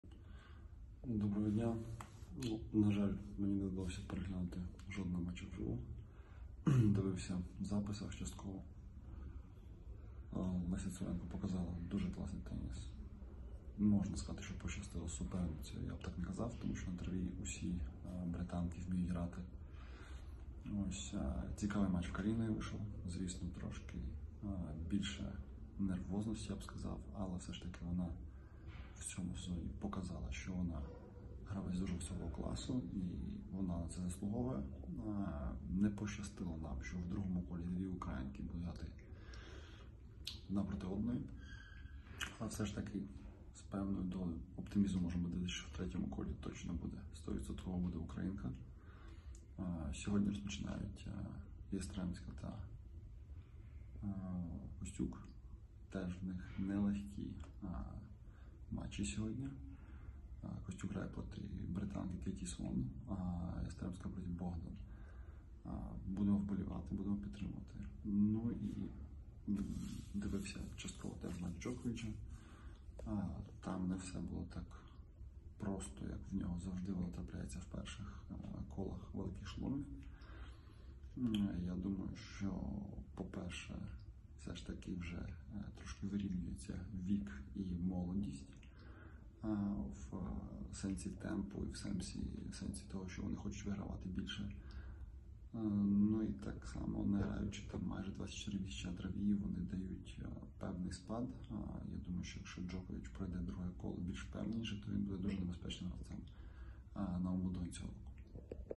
Аналіз першого ігрового дня Вімблдону від Сергія Стаховського
Колишня «перша ракетка» України Сергій Стаховський проаналізував стартові поєдинки українок Ангеліни Калініної і Лесі Цуренко в перший день Вімблдону. Він коротко проаналізував їх матчі, висловився, як ставиться до того, що жереб звів їх в другому колі змагань і пояснив, чому чинний чемпіон Вімблдону Джокович мав складний старт.